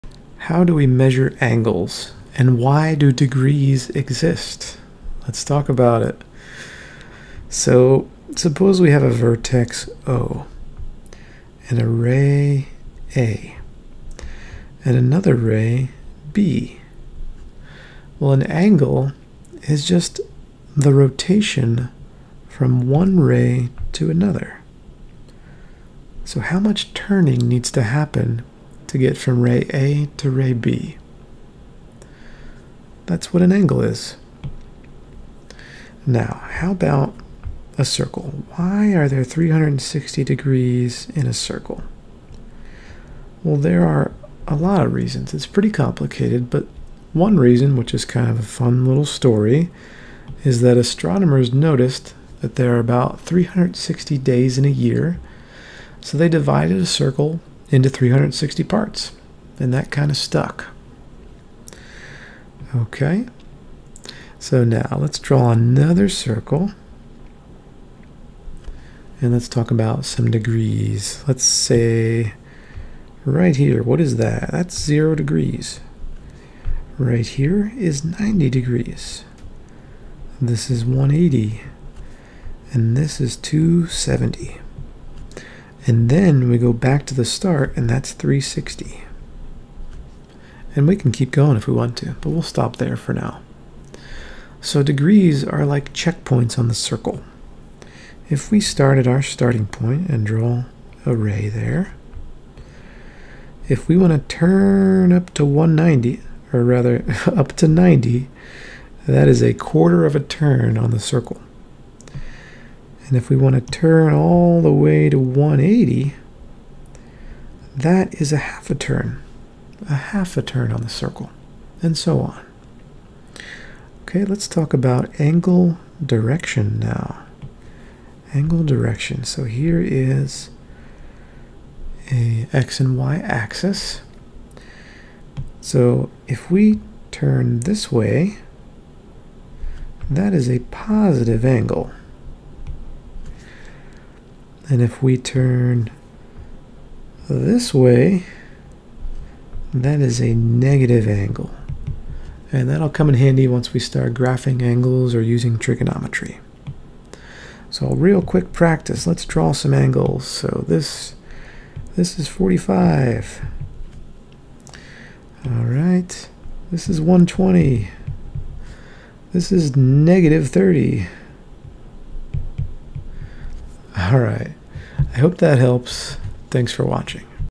What's an angle? Pre-Calculus 5.1.1 Angle measure: degrees, DMS notation, and rotation (1/2)
5. Pre-Calculus
5.1 Trigonometry: Angles, Triangles, & the Unit Circle
5.1.1 Angle measure: degrees, DMS notation, and rotation (1/2)

Welcome to the first part of this pre-calculus series! Here, we quickly review angles and degrees. An angle is a way to measure how much you need to turn to get from one ray to another when they share a vertex. Now, why are there 360 degrees on a circle? Well, it's partly because astronomers estimated 360 days in a year. We'll also talk about positive and negative angles, and how they depend on which way we rotate.